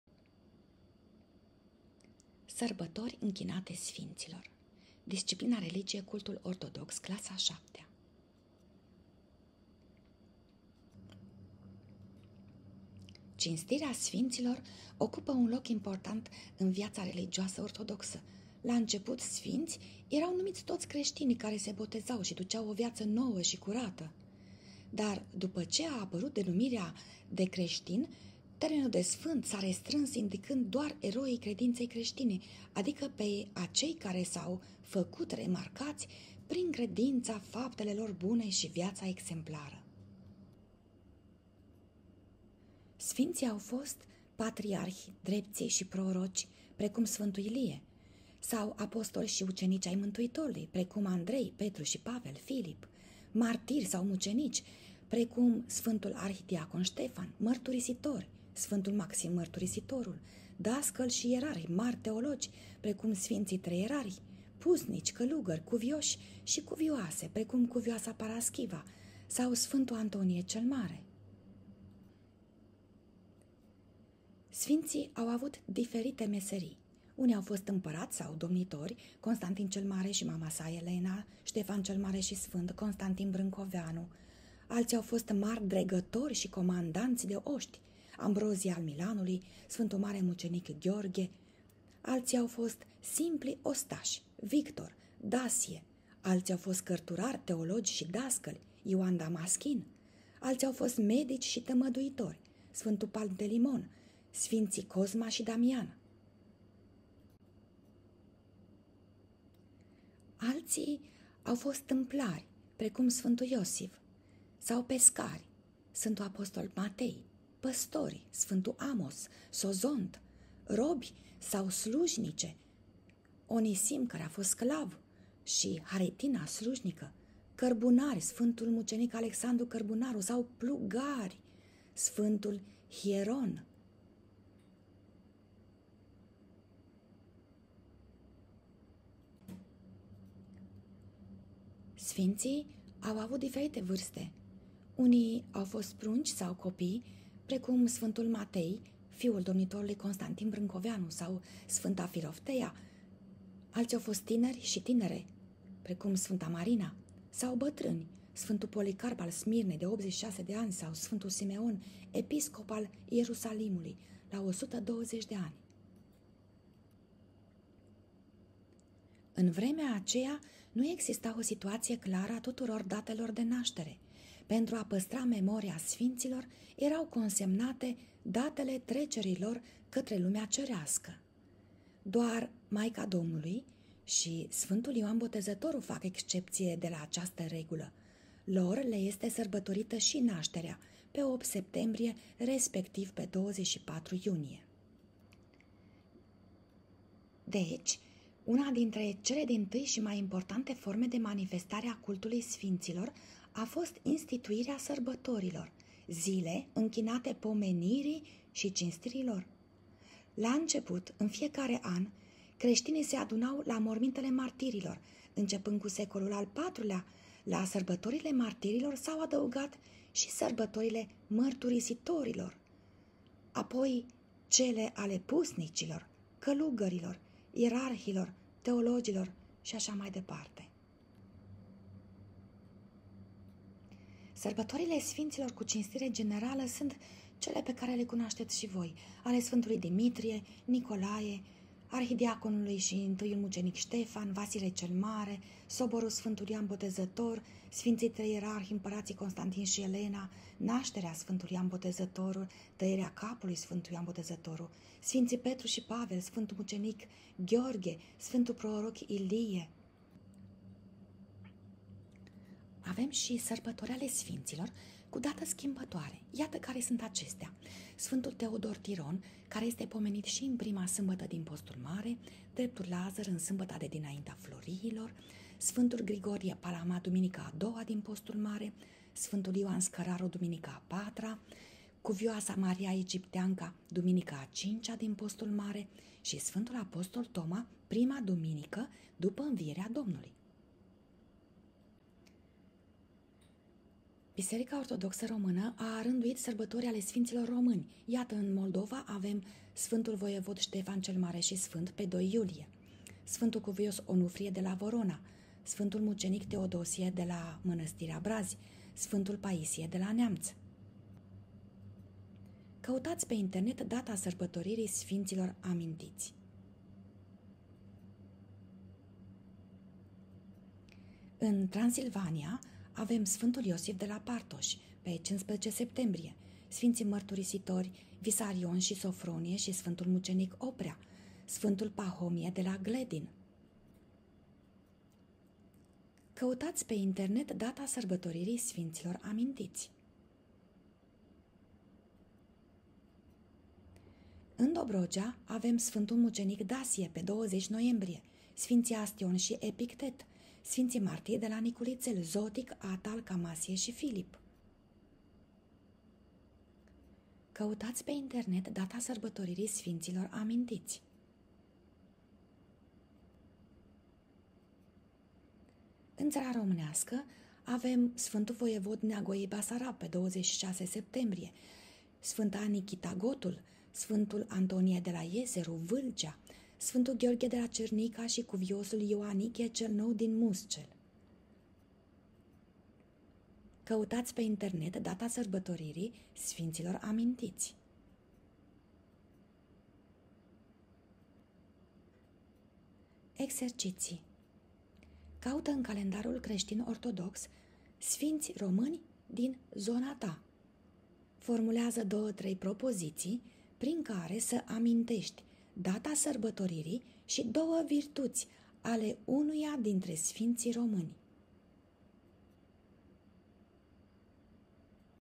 Sărbători închinate sfinților
Resursa conține informații despre sărbătorile închinate sfinților însoțite uneori de icoana sfinților. Resursa conține exerciții de identificare a datei sărbătorilor sfinților amintiți.
Resursa poate fi întreruptă ori de câte ori profesorul consideră că este necesar. De asemenea, în funcție de nivelul clasei, resursa poate fi vizionată complet sau parțial.
Scopul urmărit de resursa propusă:
- este de a ajuta elevul să-și însușească informații despre sărbătorile închinate sfinților;
- oferă oportunitatea descoperirii icoanelor praznicare închinate sfinților;
- ajută parcurgerea drumului către descoperirea unor aspecte de diversitate legate de sărbătorile închinate sfinților.
Concepte cheie: sfinți, sărbătoare, icoană.

Autor: Adriana Nicoleta Argatu